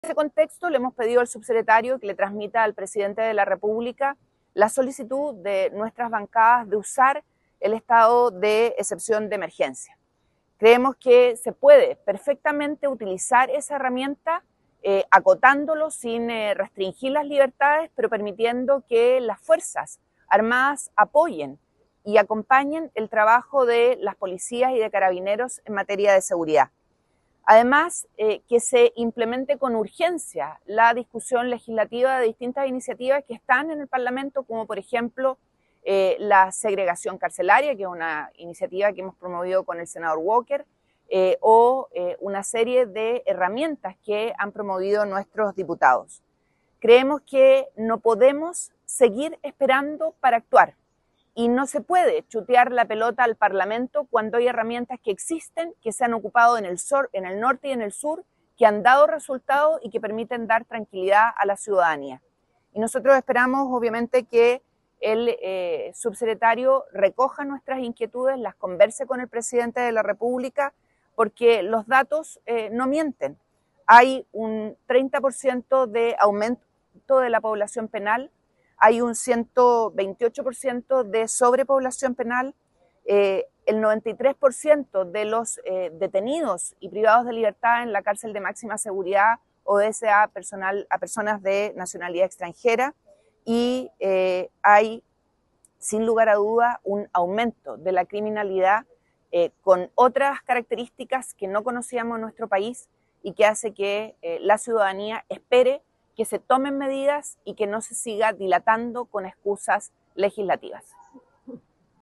Propuesta integral de Seguridad 2207 Punto de prensa Demócratas
Hoy, desde La Moneda, como parlamentarios de @democratas, entregamos al Subsecretario de Interior, Manuel Monsalve, una nueva propuesta integral de seguridad.

La propuesta incluye medidas como:
•⁠  ⁠Segregación en cárceles entre criminales organizados y delitos menores.
•⁠  ⁠Creación de una Unidad de Inteligencia Regional.
•⁠  ⁠Control preventivo para migrantes.

Estas medidas se suman a las pendientes, como el control fronterizo terrestre por parte de las FFAA y el estado de excepción en la RM.

Desde Demócratas, reafirmamos nuestro compromiso en la lucha contra la delincuencia y el crimen organizado.